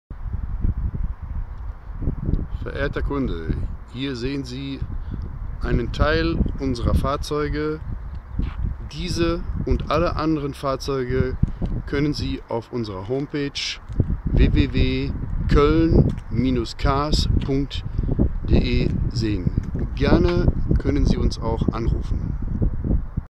Renault Clio GT III 1.2 16V Navi Klima 24.tkm GA von Coeln-Cars, Dirk Kleimann
Renault Clio GT III 1.2 16V Navi Klima 24.tkm GARANTIE von Coeln-Cars, Dirk Kleimann mit 24.000km weitere Informationen und Fahrzeuge